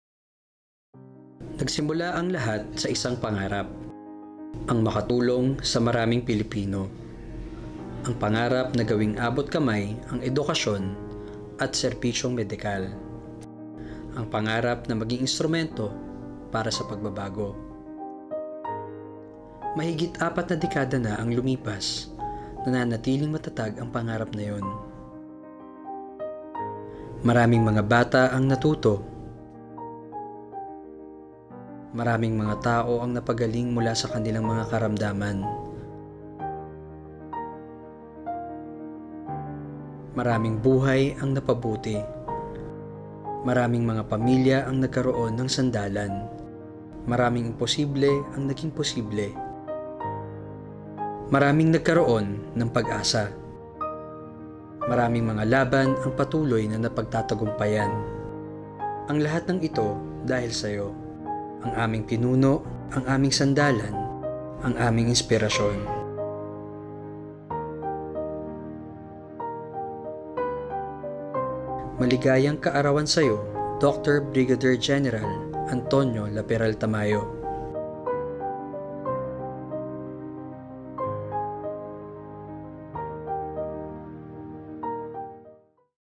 The Legacy of DR / BGEN Antonio Laperal Tamayo
Happy 75th birthday to our Founder, Chairman of the Board and CEO, DR/BGEN ANTONIO L TAMAYO AFP FPCHA PHD. On behalf of Perpetual Help Medical Center - Las Piñas, we thank you for your great leadership, inspiration, and service to the Filipino people.